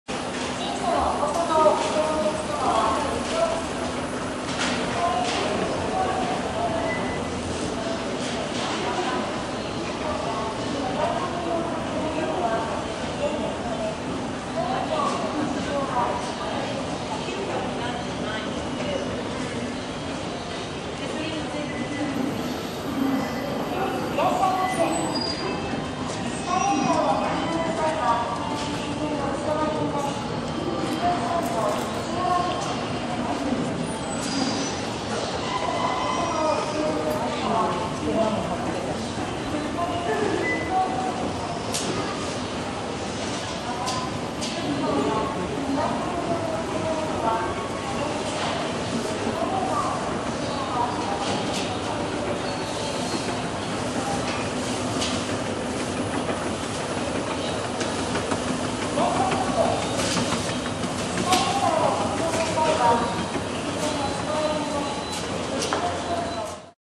二子玉川ライズ 地下駐車場 エスカレーター Escalator Futakotamagawa Rise Tokyo walking
東急田園都市線、二子玉川駅そばのショッピングセンター、
ライズのエスカレーター。東急ストア

【amazonリンク】